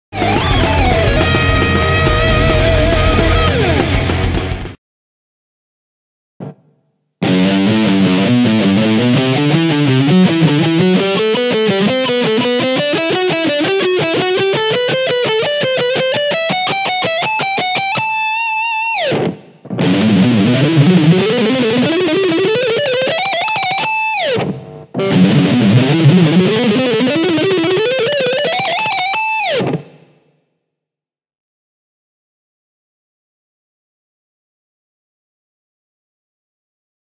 SHRED ACTION: Alternate Picking group of 5
Guitar Licks for your daily workout.

Francesco Fareri proudly uses and endorses:
ESP GUITARS / EMG PICKUPS / GHS STRINGS